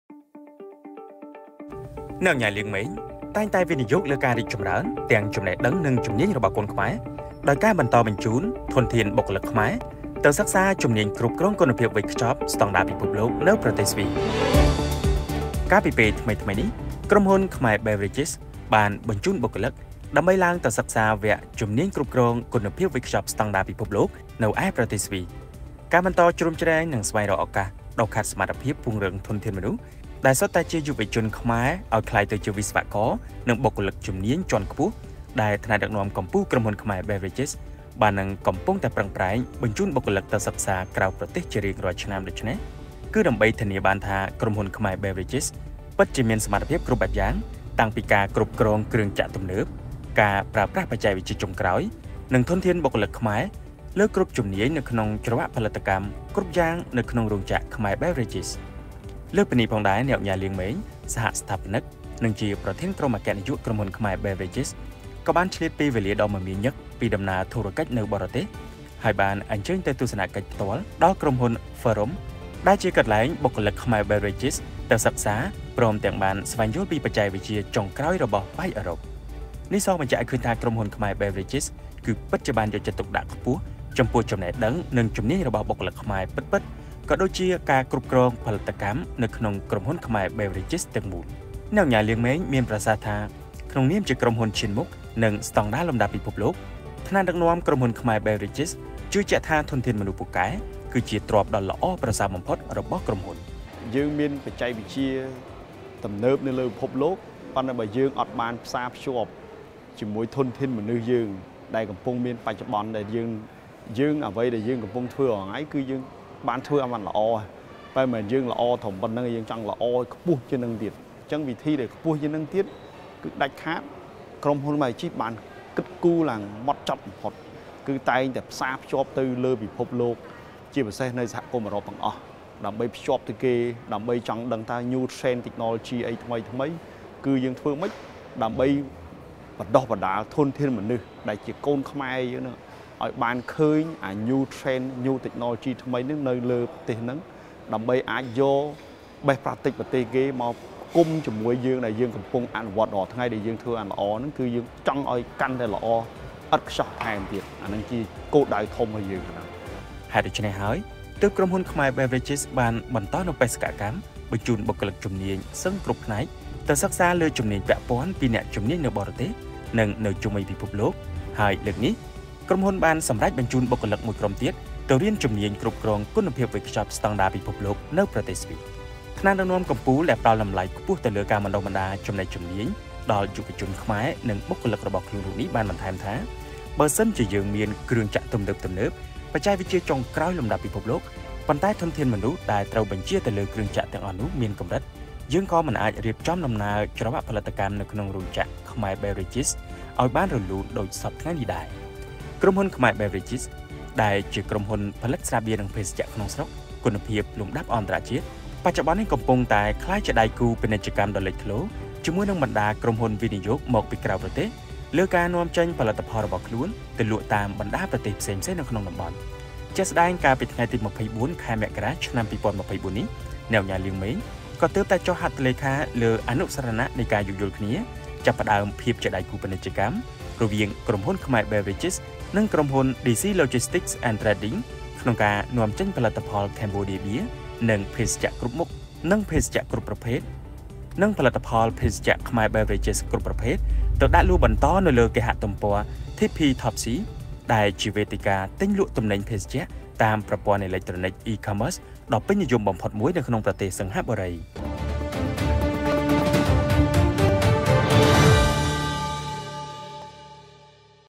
អ្នកឧកញ៉ា លាង ម៉េង តែងតែវិនិយោគលើការរីកចម្រើនទាំងចំណេះដឹង និងជំនាញរបស់កូនខ្មែរ
ដោយការបន្តបញ្ជូនបុគ្គលិកខ្មែរ ទៅសិក្សាជំនាញគ្រប់គ្រងគុណភាពវេចខ្ចប់ស្តង់ដារពិភពលោក នៅប្រទេសស្វីស
តាមដានគេហទំព័រឧកញ៉ាញូស៍ ដំណឹងគ្មានដែនកំណត់